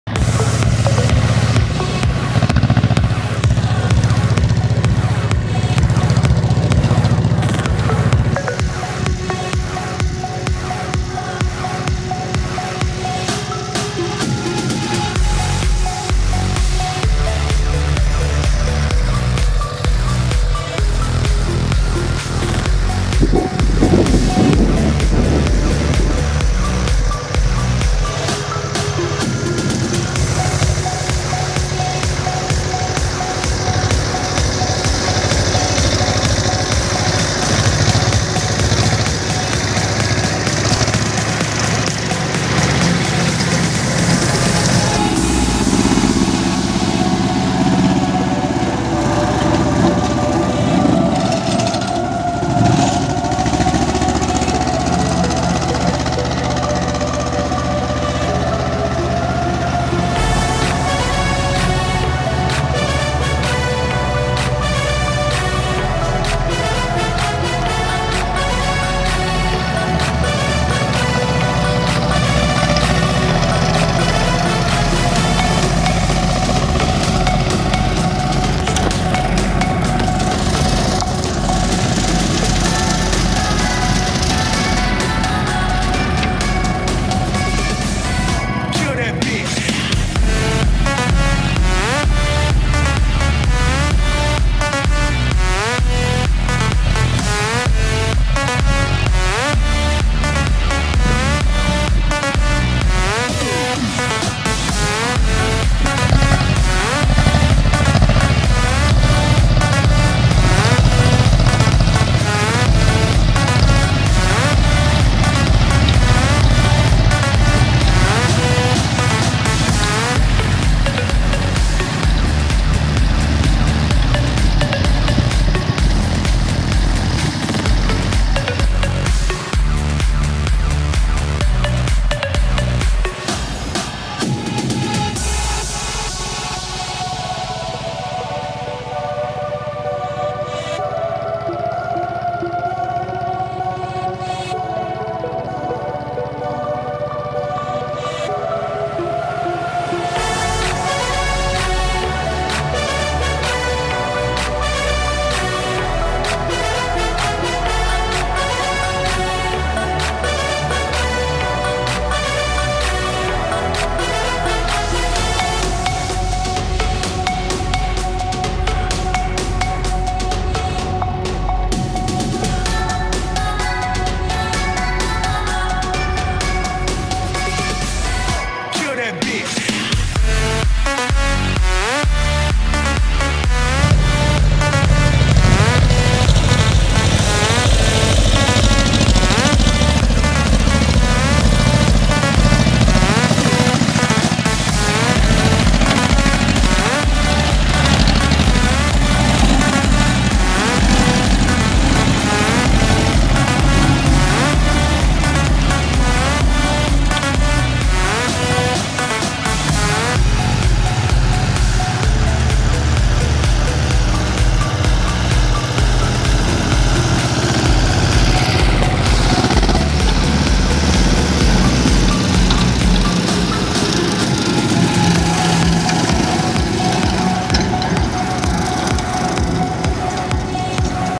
Jesienne koszenie malin. Ursus c-330M [FULL HD]
Witam w tym filmie było pokazane koszenie malin kosiarką Rokosan Femarol Słupsk 1,6m. Mam nadzieje że film się spodoba/spodobał jak tak to bardzo proszę o łapkę w górę oraz subskypcję i napiszcie komentarz.